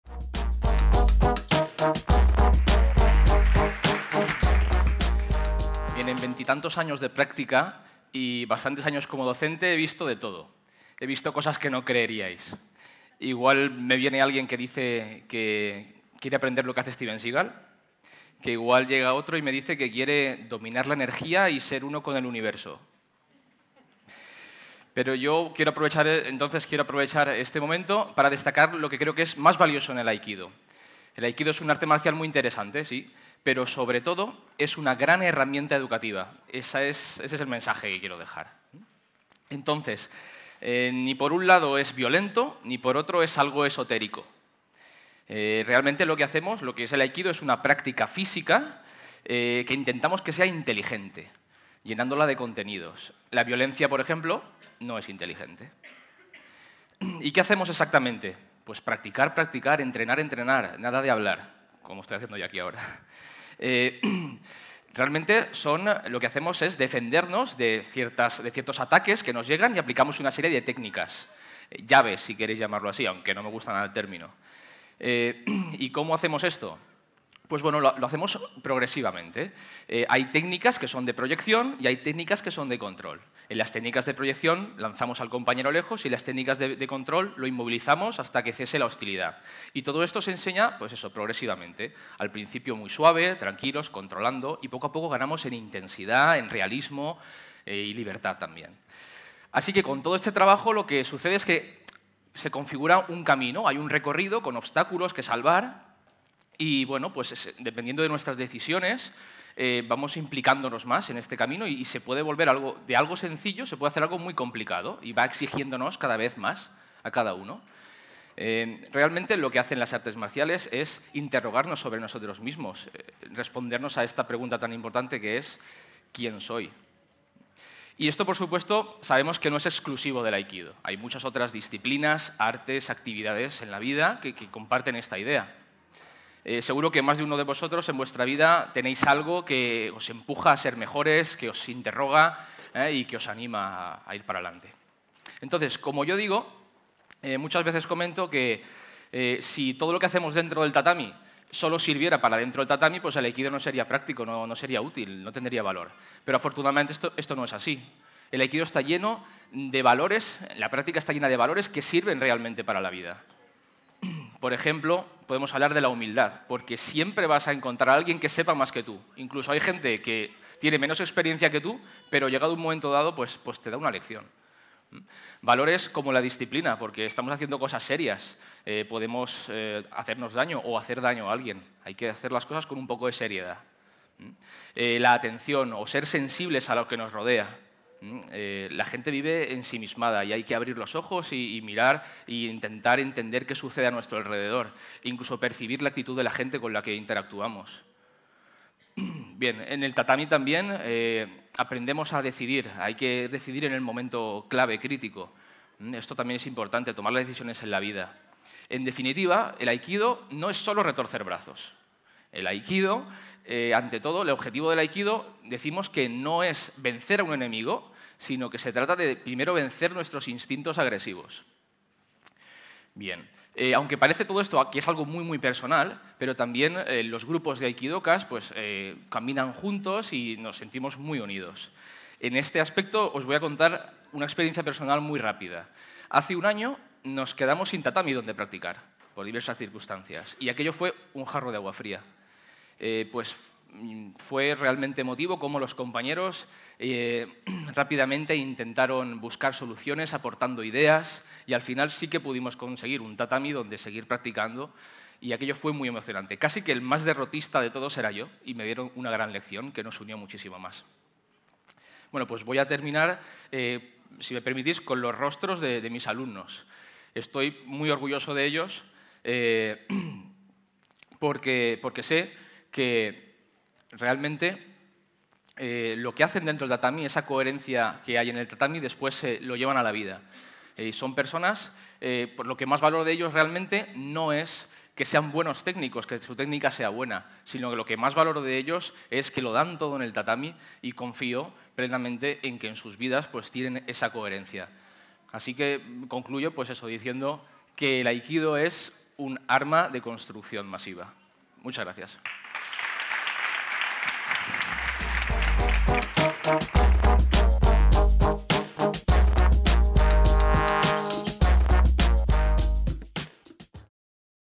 Ignite Valencia #4 - Víctor Gutierrez: El Aikido, arma de construcción masiva
Ignite Valencia #4 se celebró el 27 de Junio de 2013.